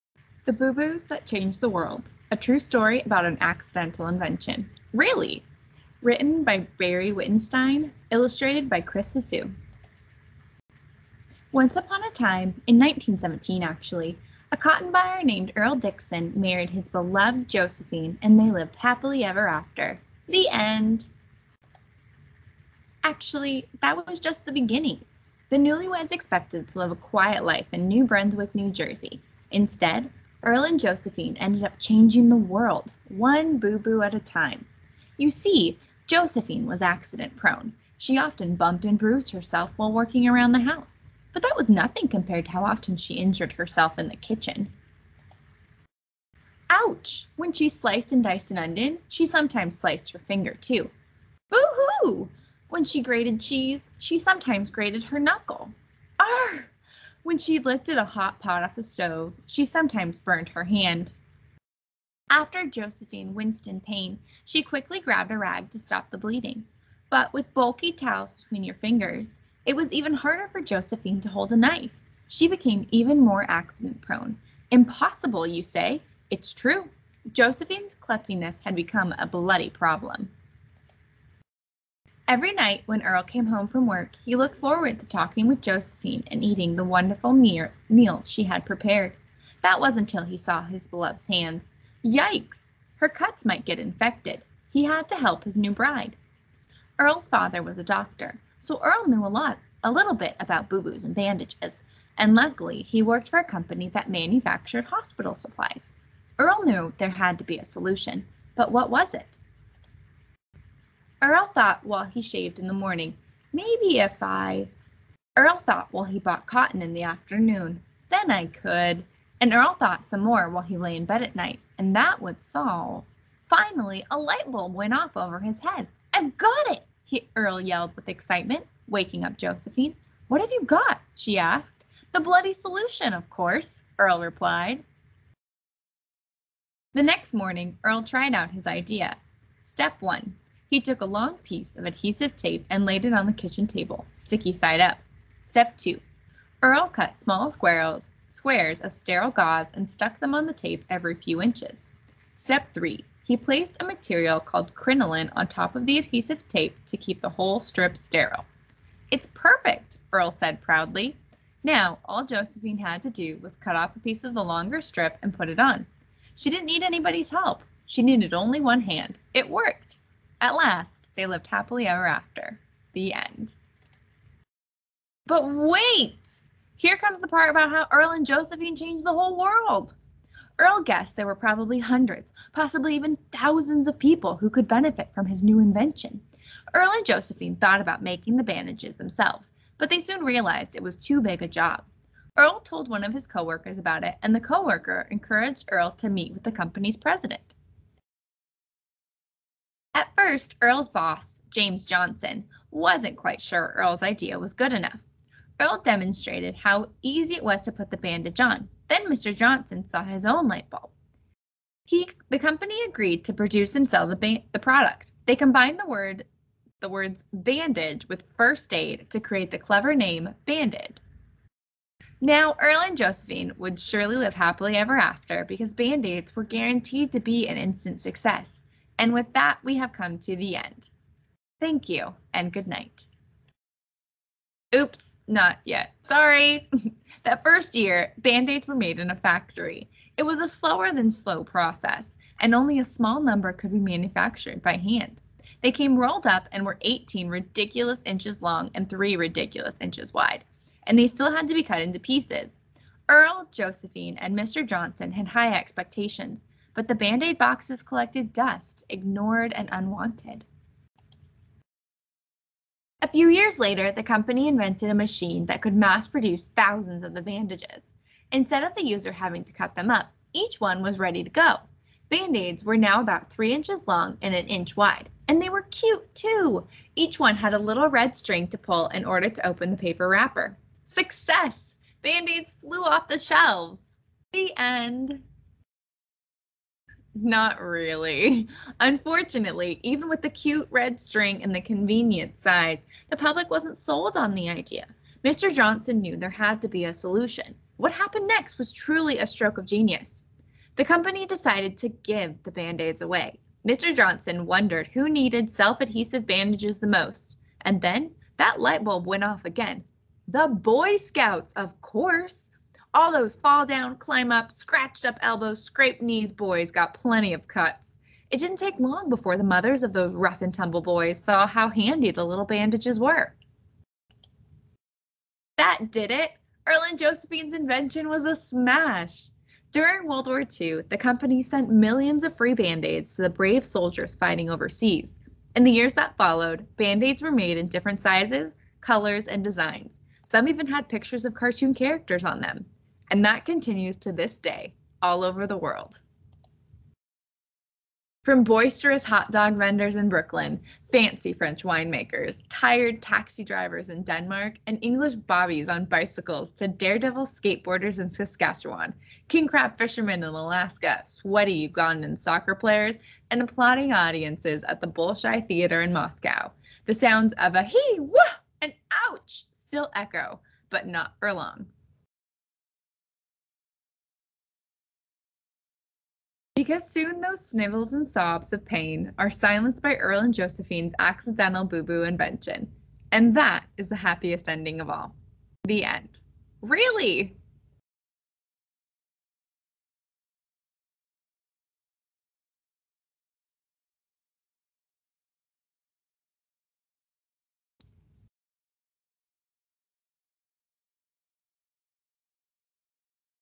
The Boo Boos That Changed the World - Read Aloud
The Boo Boos That Changed the World: A True Story About an Accidental Invention (Really!) - Read Aloud

From Amazon: 1920s cotton buyer Earle Dickson worked for Johnson & Johnson and had a klutzy wife who often cut herself. The son of a doctor, Earle set out to create an easier way for her to bandage her injuries. Band-Aids were born, but Earle's bosses at the pharmaceutical giant weren't convinced, and it wasn't until the Boy Scouts of America tested Earle's prototype that this ubiquitous household staple was made available to the public. Soon Band-Aids were selling like hotcakes, and the rest is boo-boo history.

Written by Barry Wittenstein
Illustrated by Chris Hsu

Digital version of book found on getepic.com